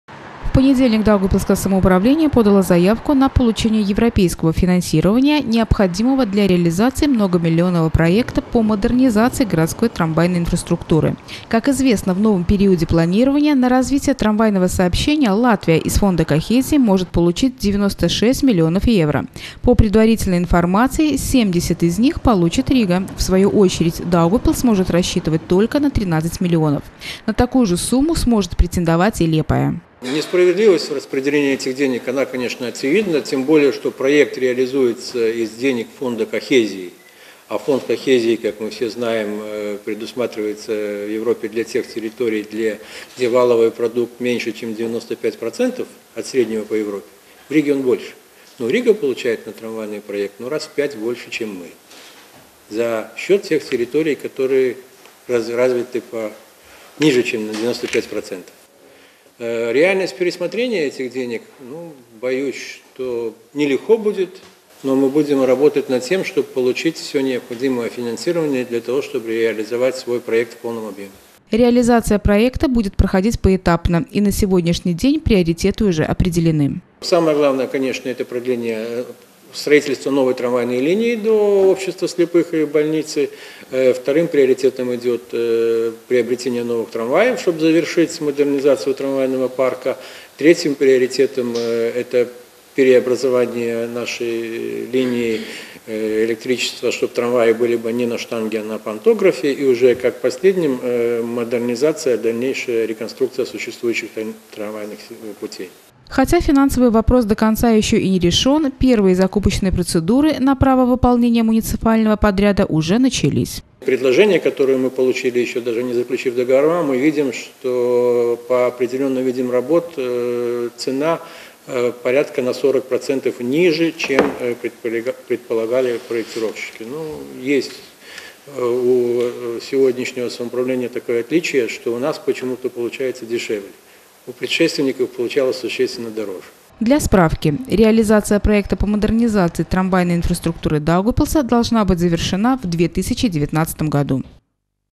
Модернизация трамвайной инфраструктуры в Даугавпилсе будет продолжена
На реализацию многомиллионного европейского проекта по модернизации трамвайной городской инфраструктуры Даугавпилс планирует получить не менее 13-ти миллионов евро. Рассказывает Лариса Кирилова.